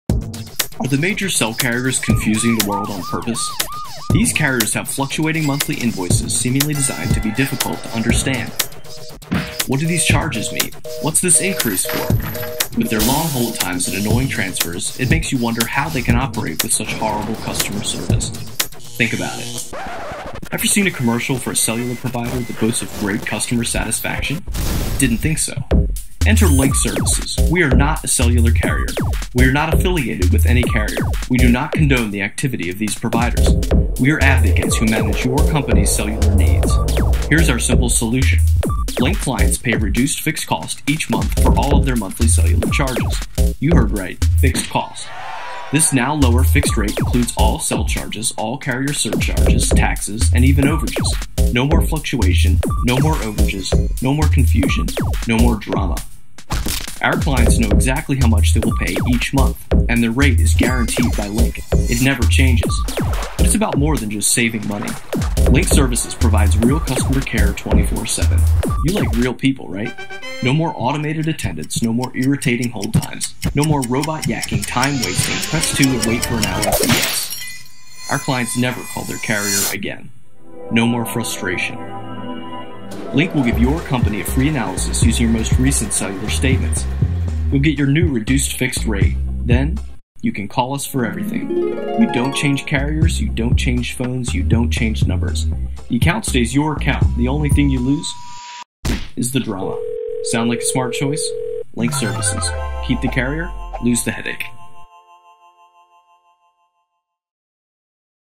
LINQ Services / Keep The Carrier, Lose The Headache
Tired of the unpredictable costs, erratic expenses and poor customer service from your cellular carrier?  Linq Services intervenes to manage your cellular service without your company needing to change carriers.  Our tagline says it all, "Keep The Carrier, Lose The Headache."  Watch this short video to see what we do in a nutshell and get rid of the cellular drama in your life.